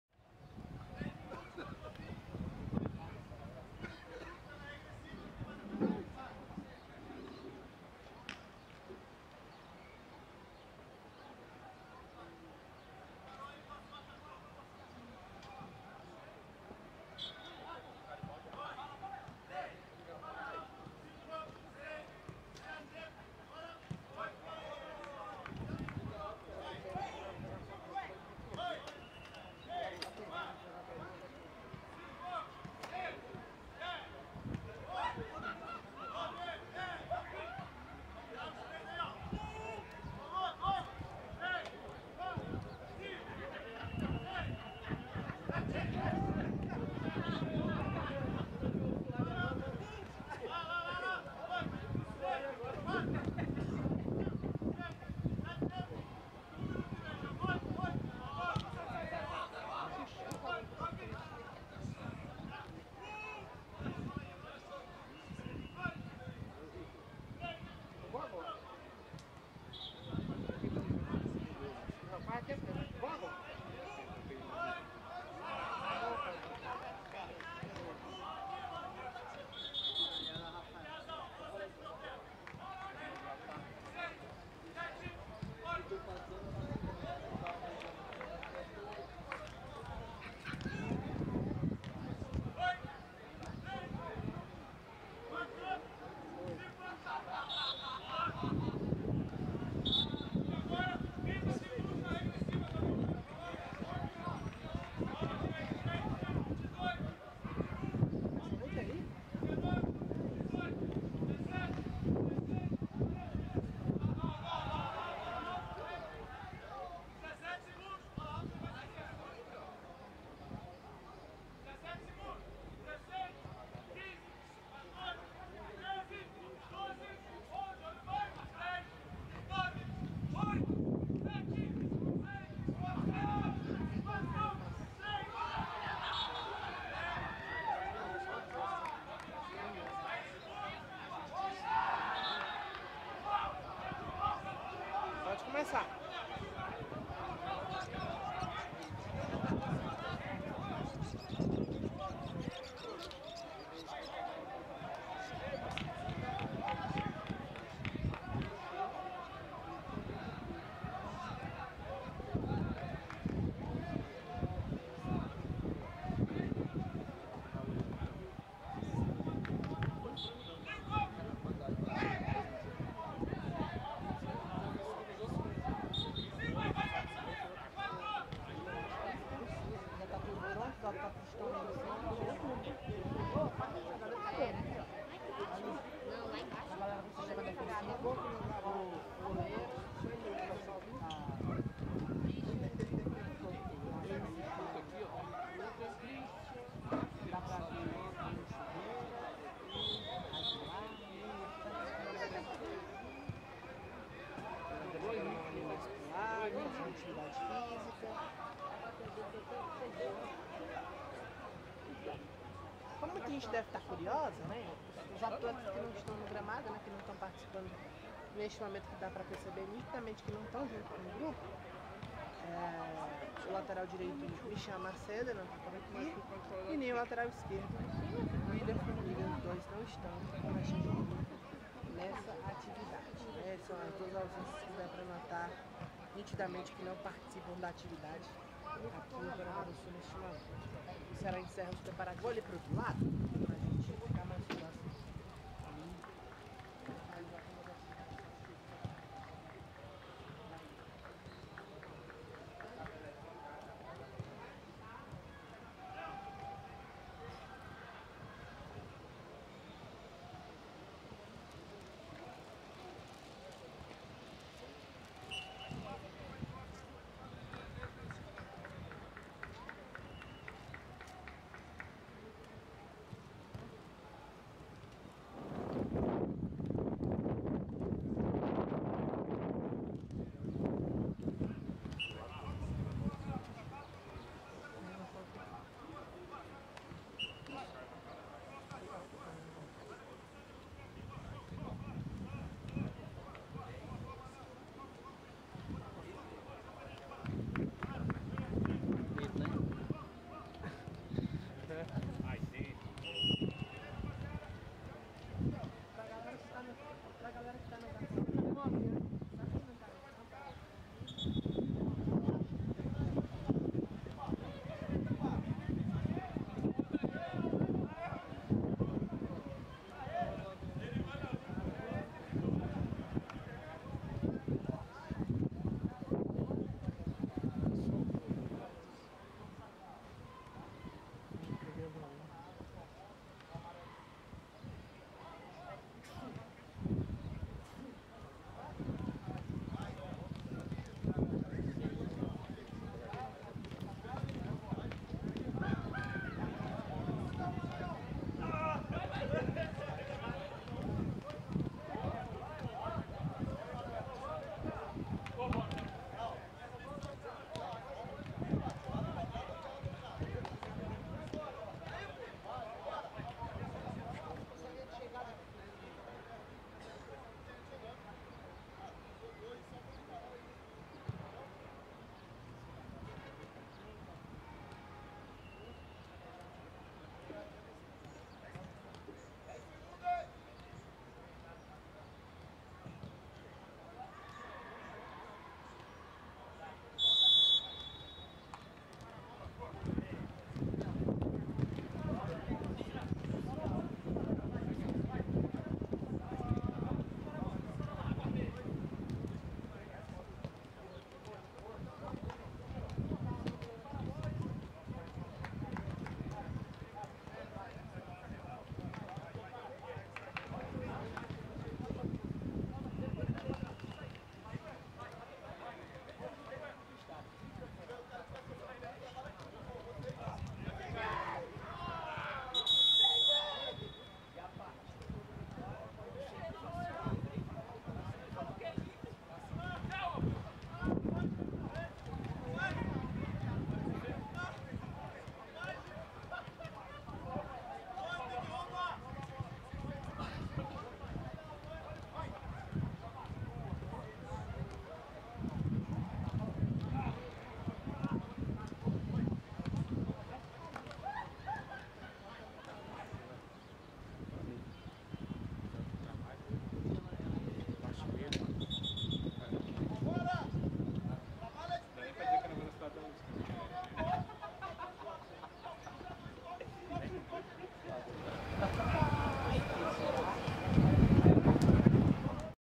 TREINO PRÉ-JOGO CEARÁ SC X VITÓRIA | 09.05.2023 | CANAL DO VOZÃO
[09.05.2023] Confira parte o treinamento de preparação para a partida Ceará SC X EC Vitória, no gramado do Vovozão!